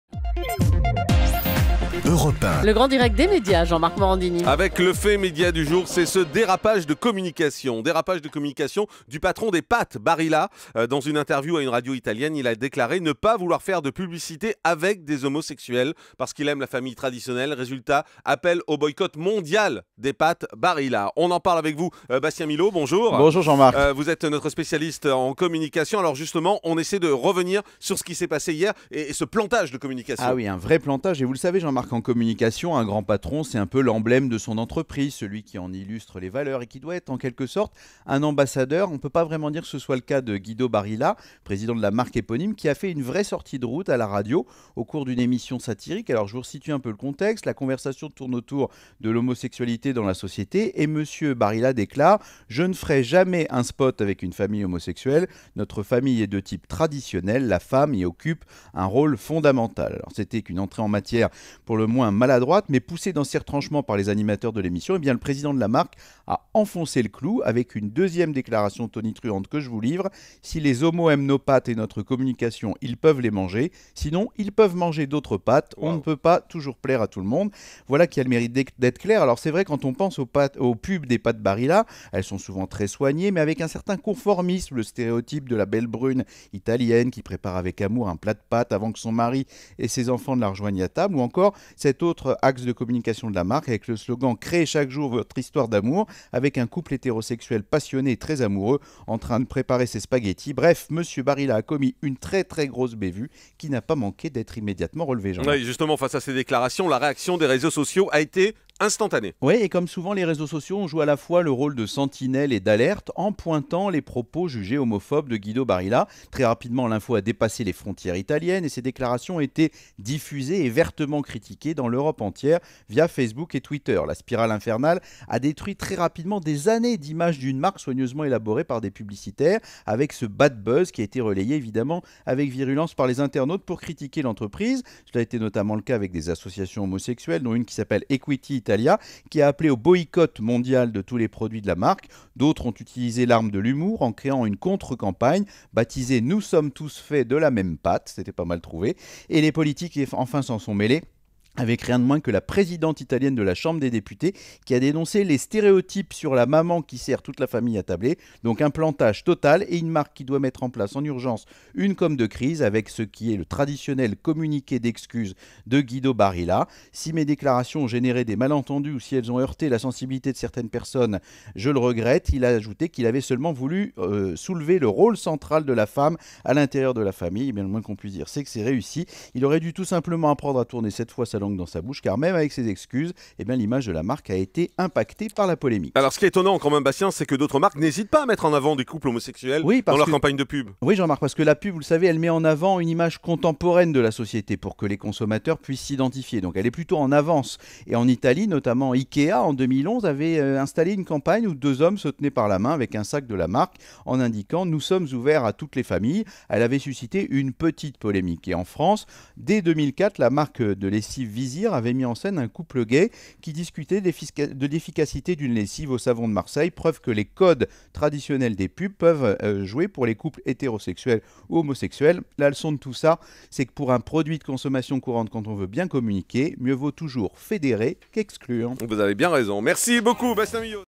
La grosse bévue de Mr Barilla
Guido Barilla, président du célèbre groupe agroalimentaire, a fait une sortie de route remarquée en expliquant qu'il ne souhaitait pas faire appel à des homosexuels pour ses publicités.